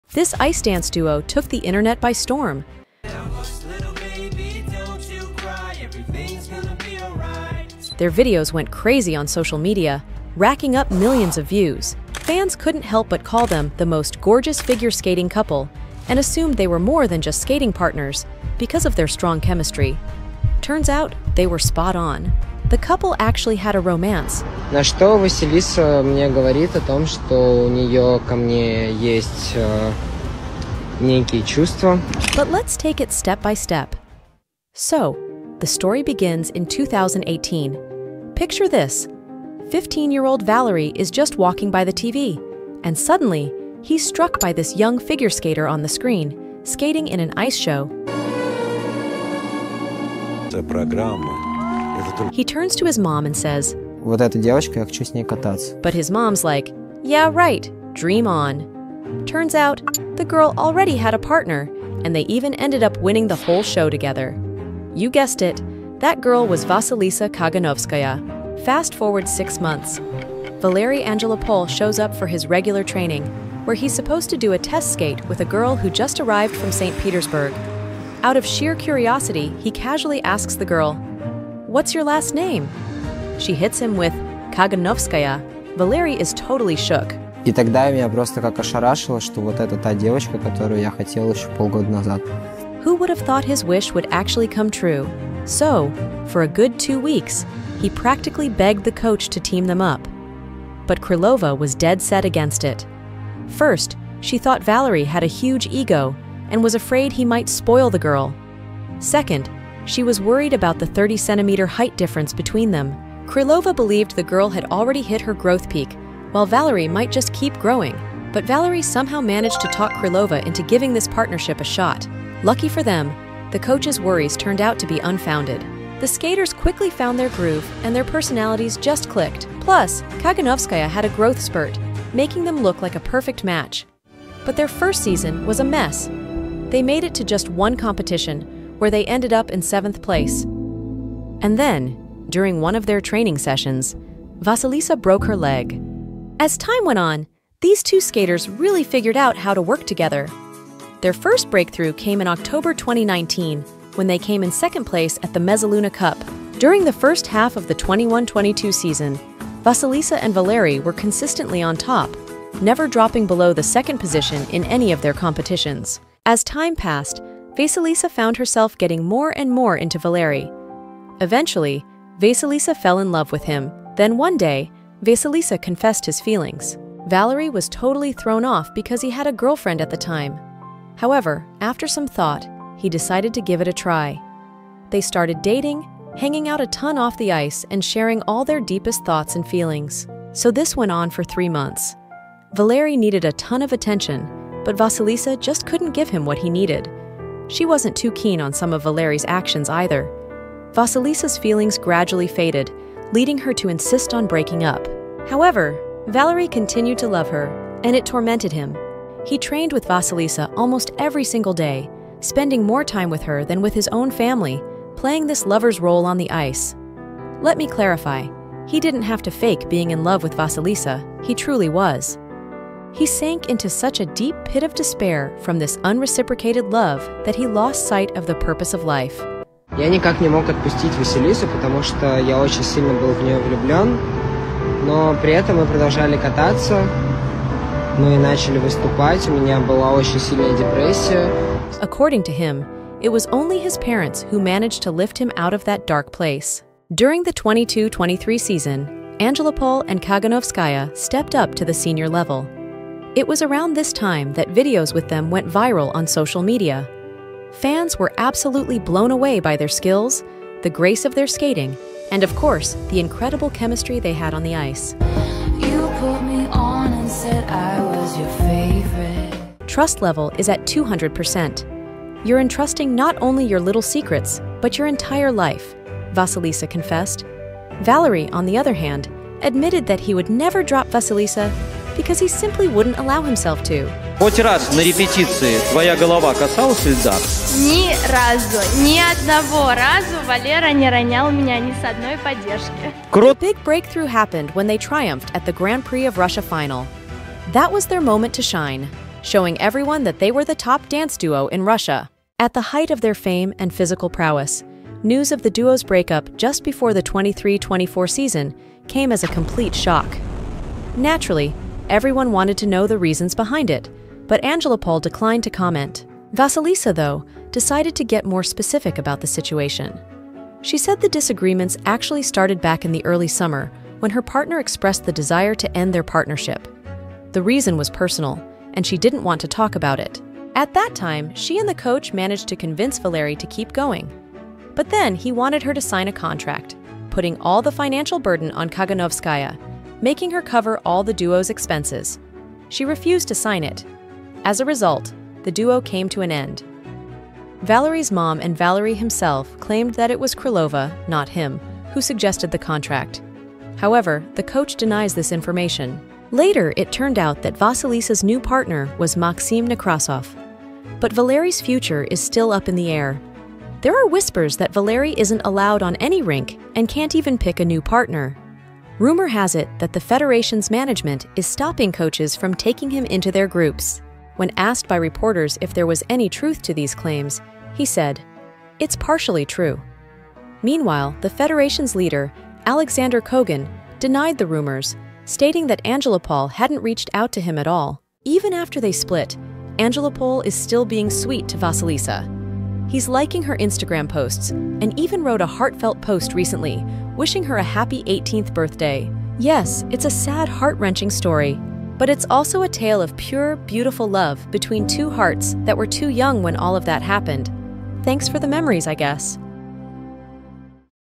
Guide to Vasilisa and Valery (drama)
I tried my best to gather every bit of information available on the internet, and I hope you found it insightful. Interestingly, after finishing the video, my perspective on Valery totally shifted. It seems to me that the contract and his complaint about Vasilisa "beating" him (I mean, really, "beating"? :D) were just excuses to get out of the situation. It's pretty clear he was so much in love with her he couldn't stand being around Vasilisa anymore, let alone playing her lover on the ice. What do you think?

title: TRUE story of Valery Angelopol and Vasilisa Kaganovskaya